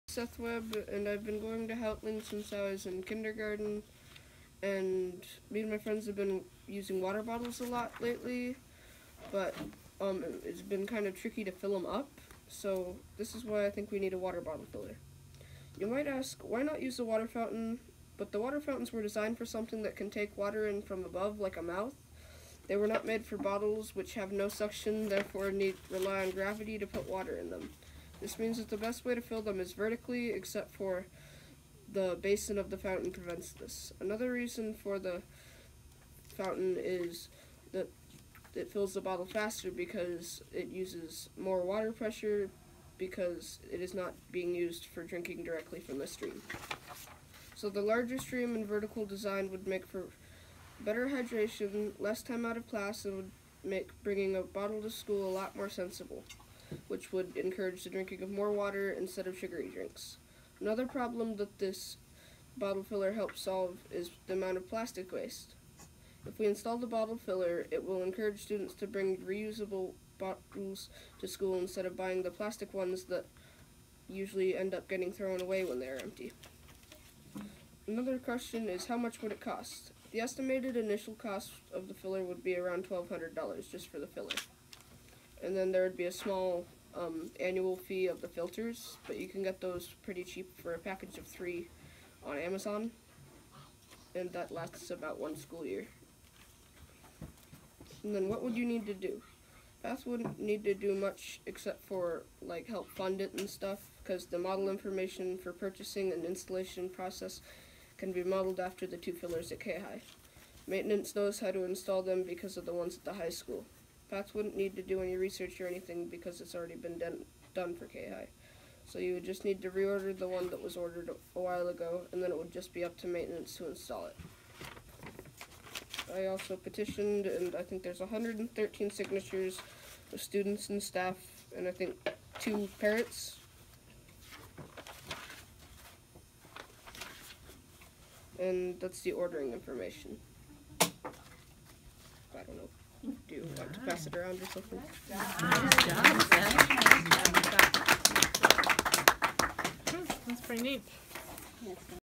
HOUGHTALING PTO
Proposing water bottle filling stations for my school